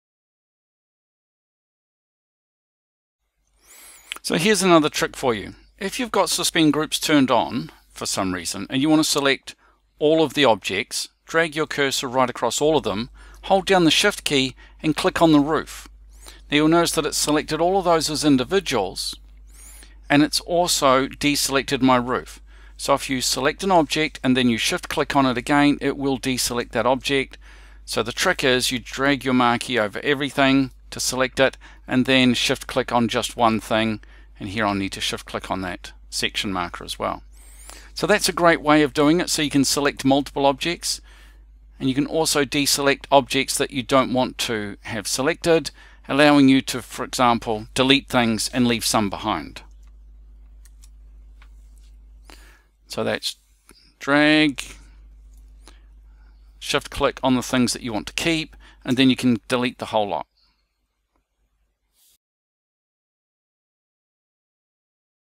Archicad - Selecting Several Objects: Shift + Click
This is a trick that I use a lot, the ability to de-select objects with Shift+Click. Many people know that you can use the technique for multiple selections. It's the same thing, but for de-selecting.